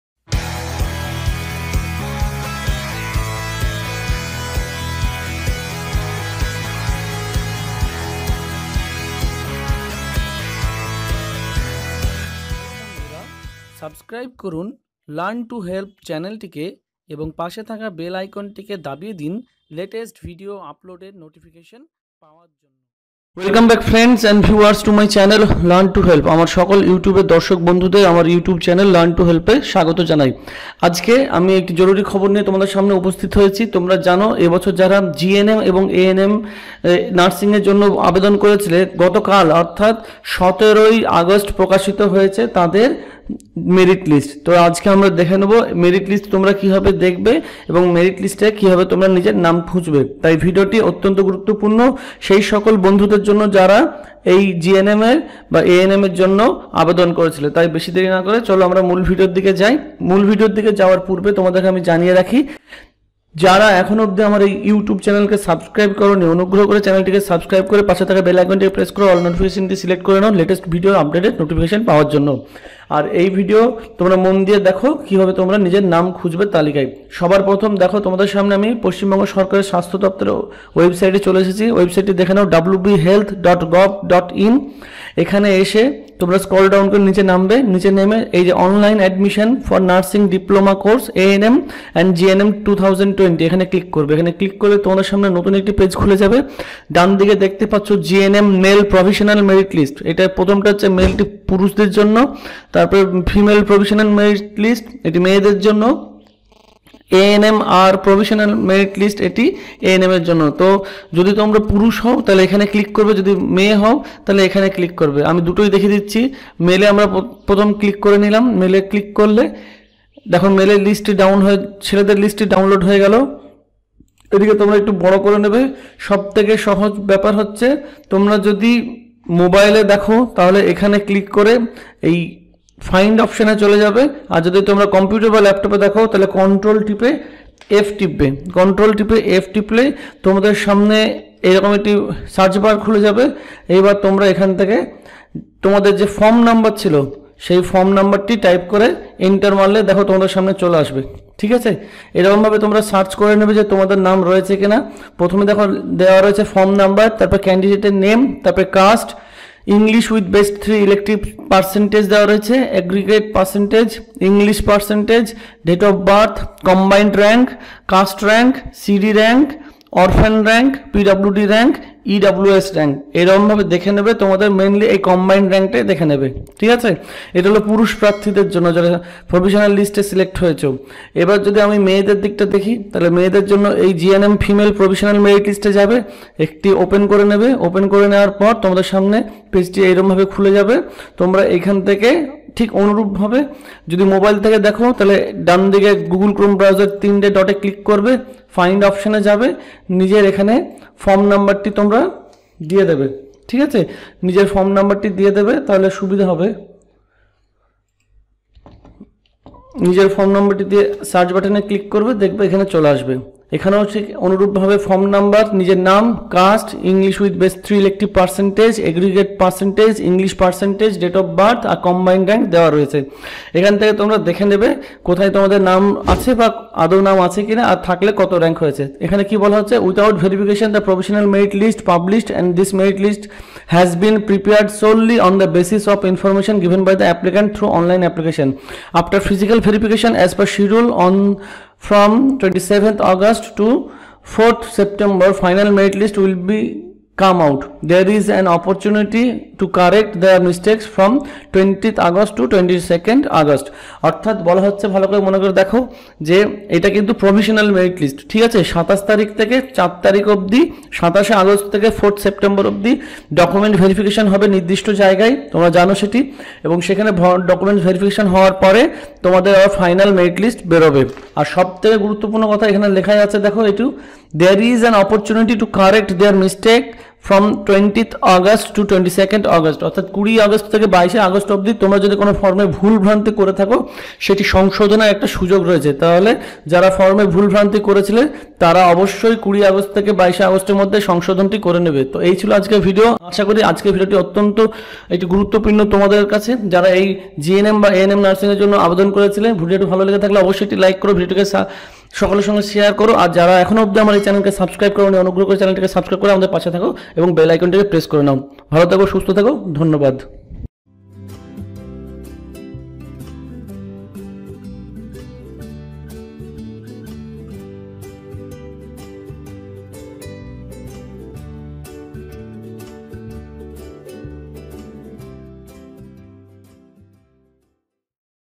প্রকাশিত হয়েছে এই বছরের GNM & ANMR এর মেধা তালিকা ।। by Learn to Help.
এছাড়াও বিভিন্ন কলেজে কিভাবে প্রথমবর্ষে অনলাইনে ভর্তির আবেদন করতে হবে সেই সংক্রান্ত অন্যান্য ভিডিও গুলির লিঙ্ক নিচে দেওয়া হলো.